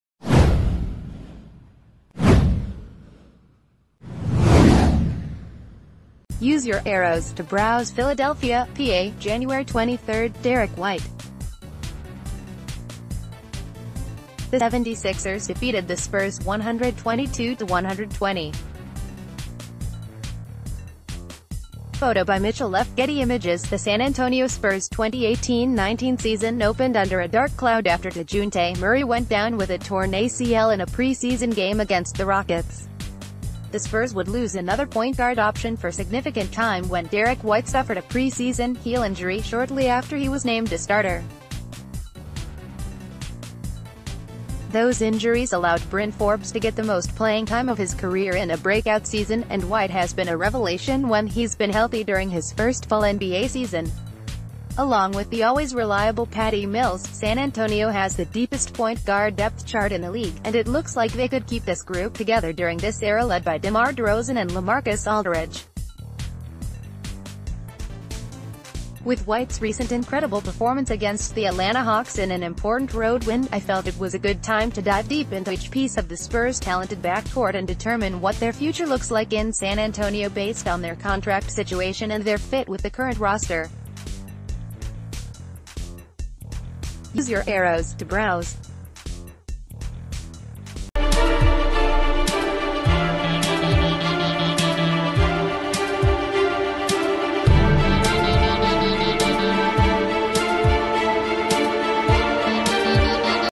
The San Antonio Spurs have the best point guard depth in the NBA
Use your ← → (arrows) to browsePHILADELPHIA, PA – JANUARY 23: Derrick White #4 of the San Antonio Spurs dribbles the ball against the Philadelphia 76ers at the Wells Fargo Center on January 23, 2019 in Philadelphia, Pennsylvania. The 76ers defeated the Spurs 122-120. (Photo by Mitchell Leff/Getty Images)The San Antonio Spurs’ 2018-19 season opened under a dark cloud after Dejounte Murray went down with a torn ACL in a preseason game against the Rockets. The Spurs would lose another point guard o...